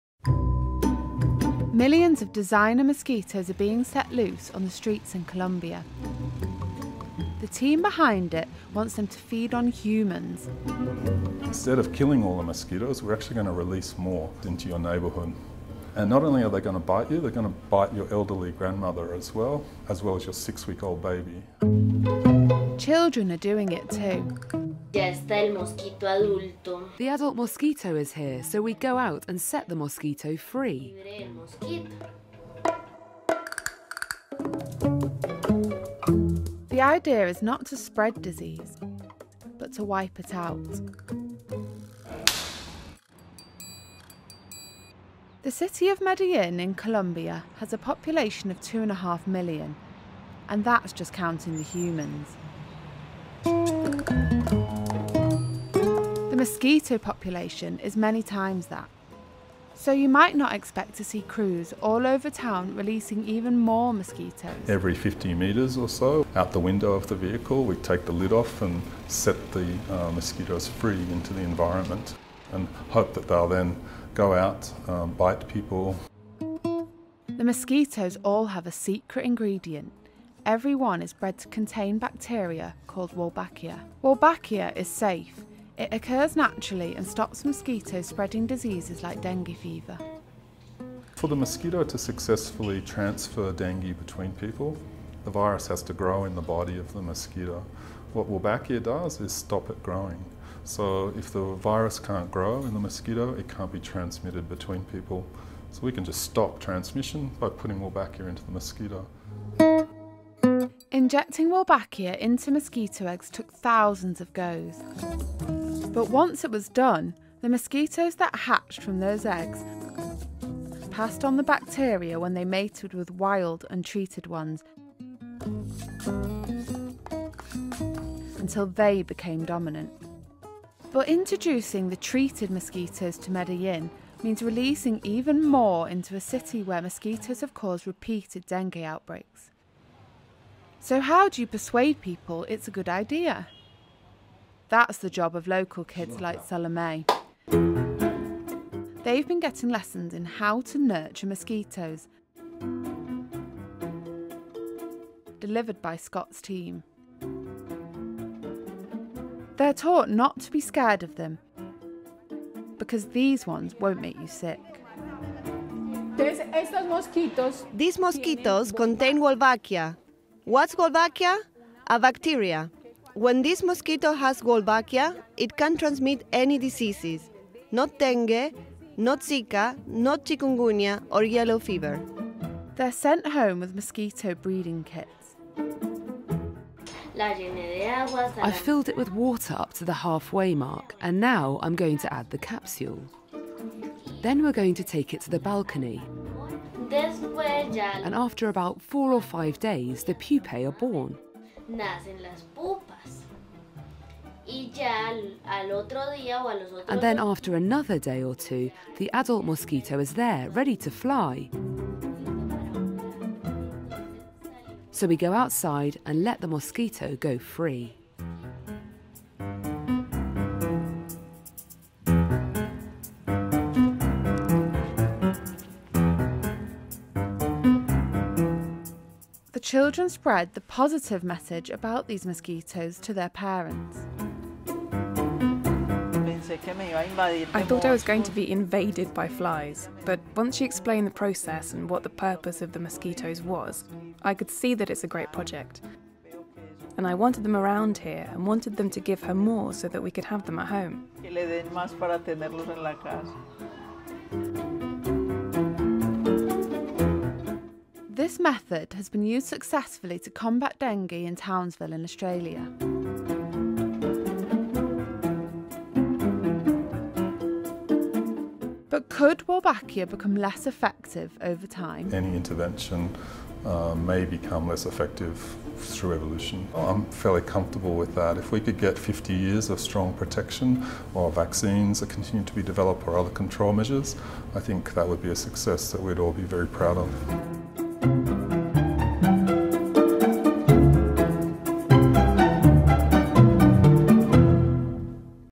The great mosquito swap - BBC News
Every year, it’s estimated that nearly 400 million people around the world are infected with dengue fever, a potentially fatal illness that’s passed on by mosquitoes.

No vaccine is effective at preventing people catching the disease, but what if the mosquitoes themselves were treated to stop them spreading it?

In one city that is severely affected – Medellin in Colombia — an ambitious project is underway to swap wild mosquitoes for a variety that is identical in every way, but with one crucial difference. These mosquitoes have been bred from specimens injected with bacteria that make it impossible to transmit not just dengue, but also the Zika and chikungunya viruses, and Yellow Fever.

Buoyed by successful projects in Australia, the World Mosquito Program is releasing millions of newly-minted mosquitoes across Medellin, in the hope that they will replace the wild population.

And to reassure the public, schoolchildren are being taught to love mosquitoes, and even to breed them — a message that contradicts what they’ve been brought up to believe.